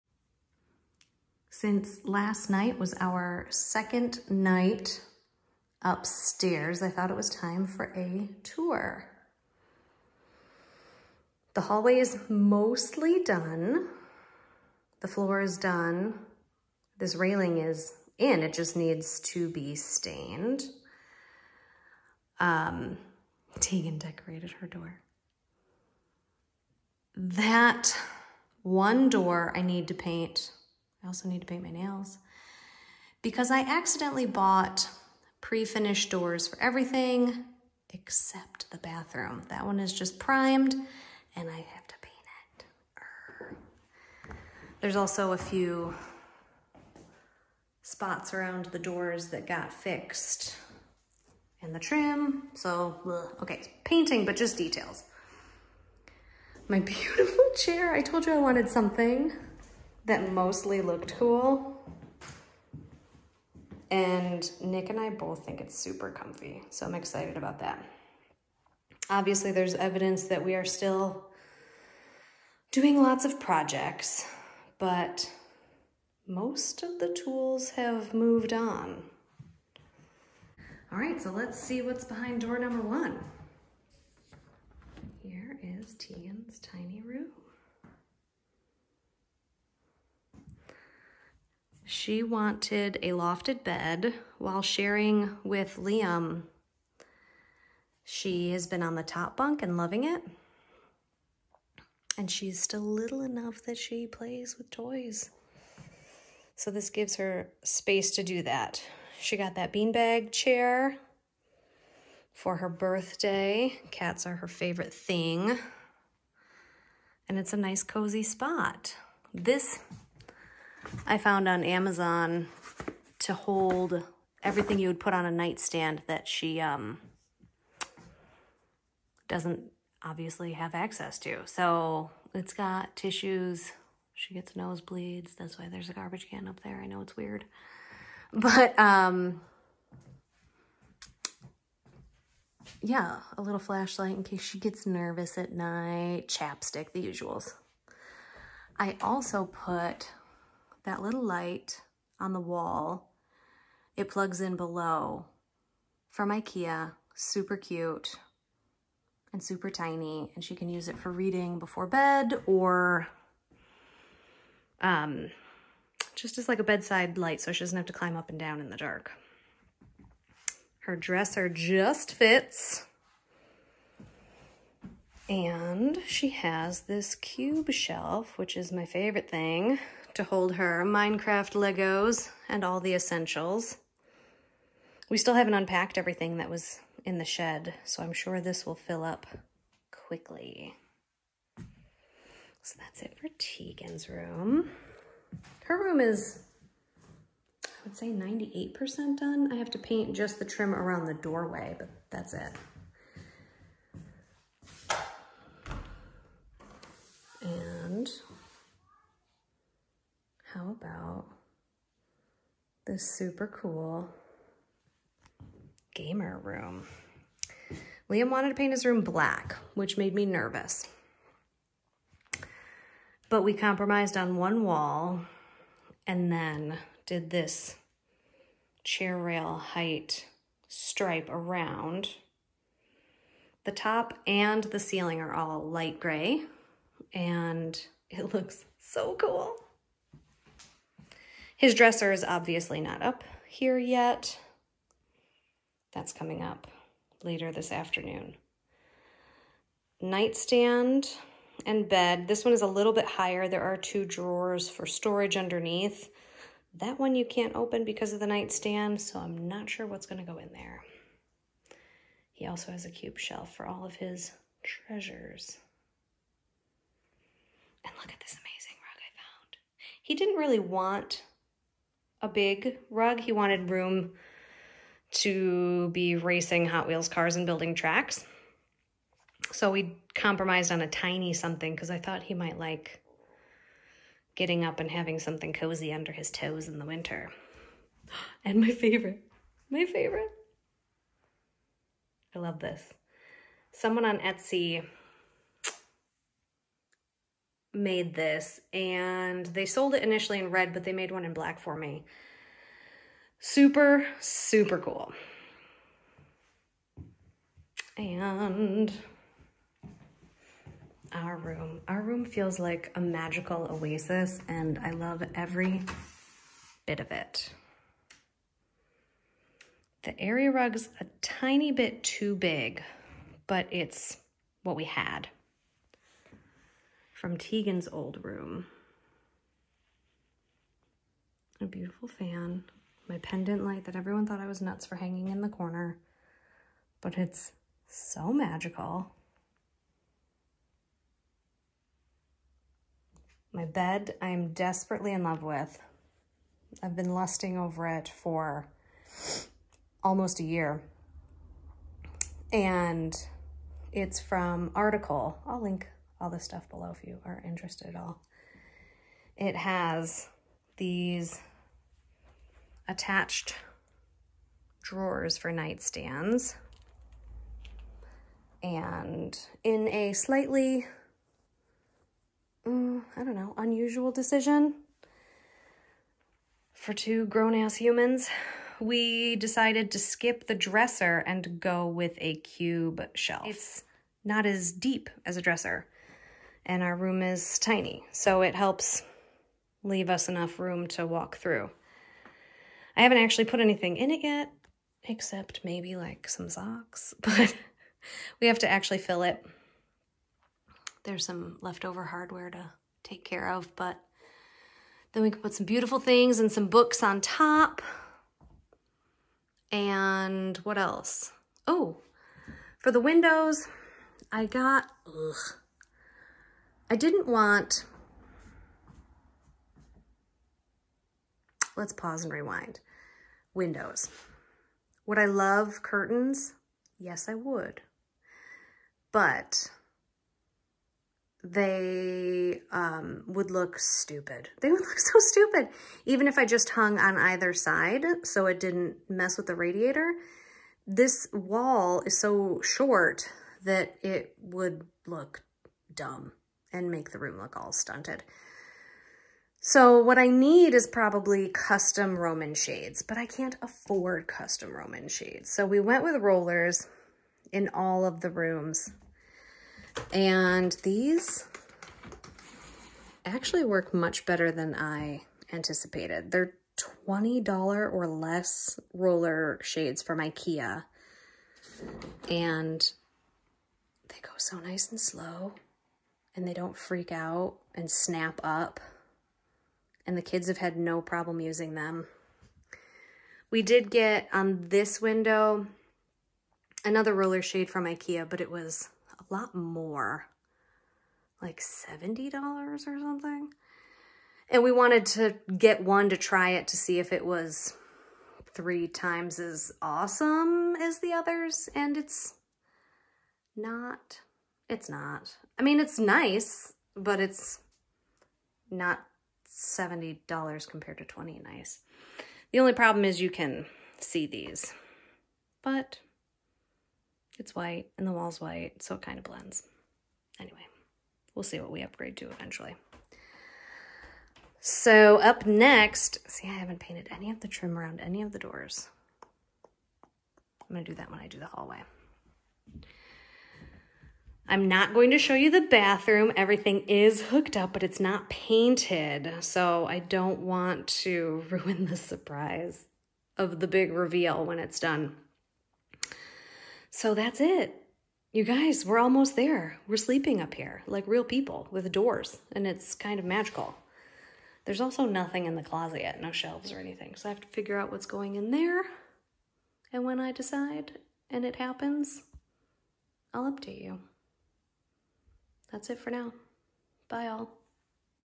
Completed Bedroom Tour | Kids Room | Main Bedroom | Bedroom Makeovers
We’re finally sleeping in our new rooms! This is THE most exciting thing. Here are a few links for the goodies in each of our rooms.

Bedside caddy

Wall lamp

Liam’s room:

Switch plate cover

Roller shades